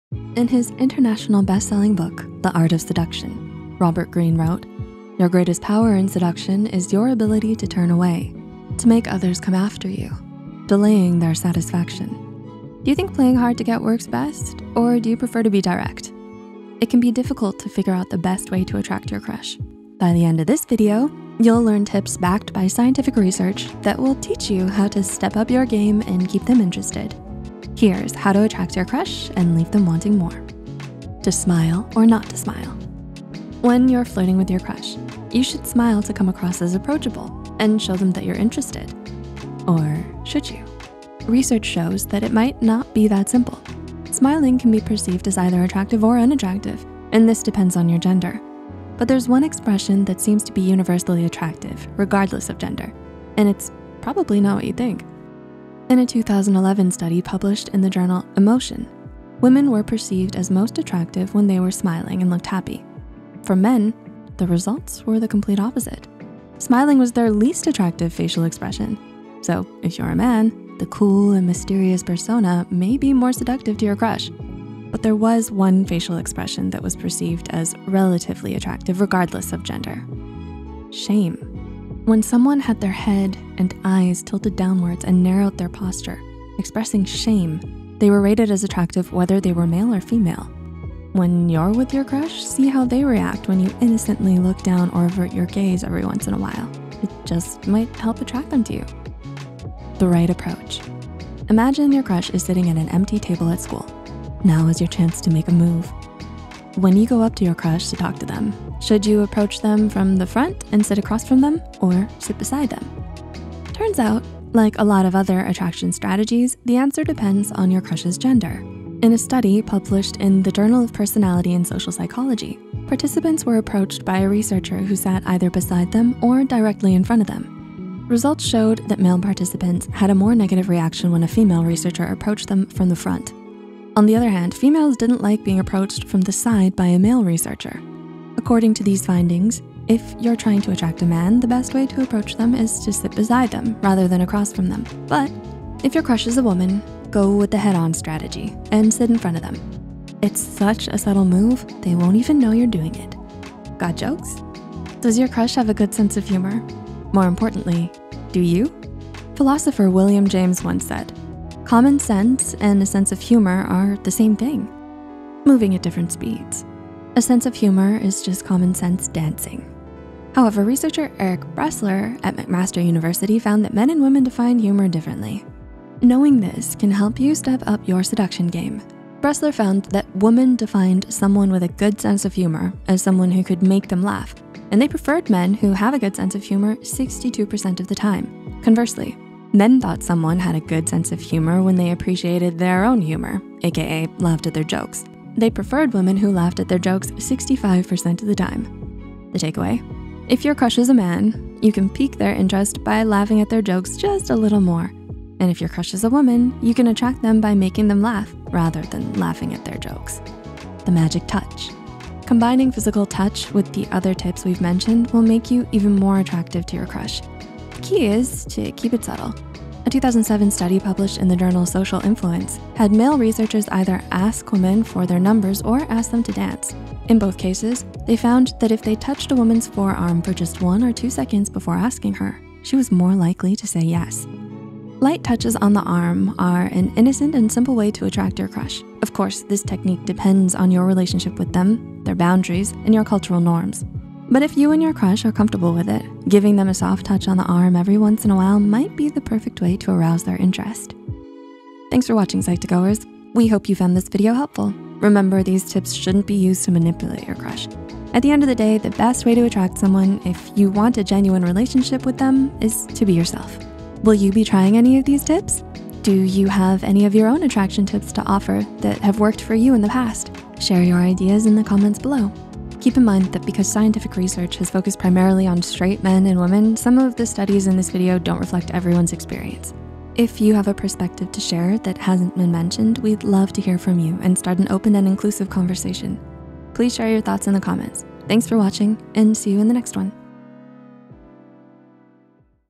How To Subtly Attract Your Crush
If you've ever wondered how to catch someone's attention without being too obvious, then you're in the right place.

In this video, we'll share practical tips and techniques to help you create a subtle and irresistible magnetism that draws your crush closer to you. We'll guide you through the art of subtle attraction, providing insights on body language, communication, and mindset. Watch this video to unlock the secrets of subtle attraction!

Writer/Researcher: Monique Zizzo
Editor: Caitlin McColl
Script Manager: Kelly Soong

References

Engle, G. (2021, February 8). Why Does Fear Make Us Horny? InsideHook.
Fisher, J. W., & Byrne, D. (1975). Too close for comfort: Sex differences in response to invasions of personal space. Journal of Personality and Social Psychology, 32(1), 15–21.
Love Me; Love My Jokes. (2005, February 5). ScienceDaily.
Tracy, J. L., & Beall, A. T. (2011). Happy guys finish last: The impact of emotion expressions on sexual attraction. Emotion, 11(6), 1379–1387.